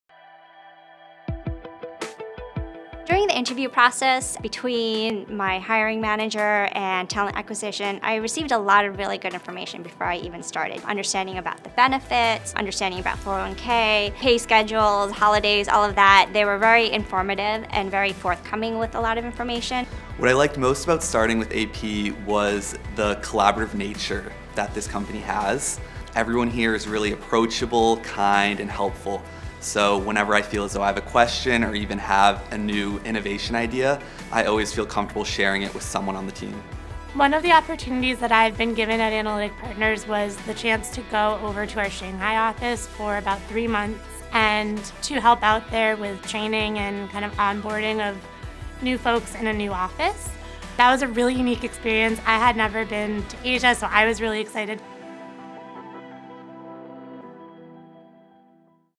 Welcome to AP - Onboarding
How important is the onboarding experience to you? Onboarding is a top priority at Analytic Partners, that’s why we use an informative approach to generate excitement and collaboration as you start.